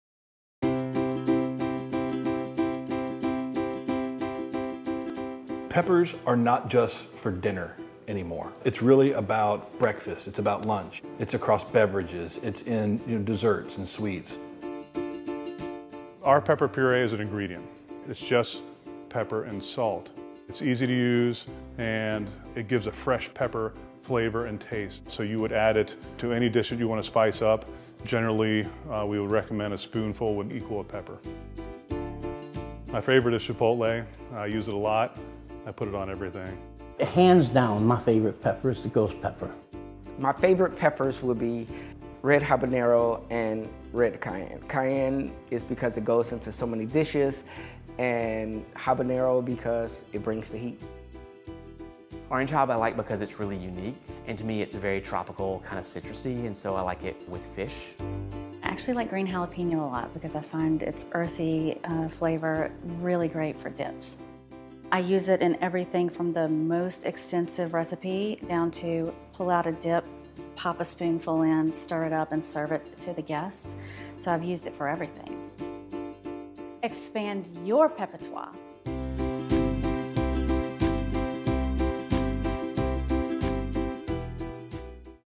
Expand Your Peppertoire!
Peppers are not just for dinner anymore! The flavor and heat of fresh chili peppers go great anytime of the day1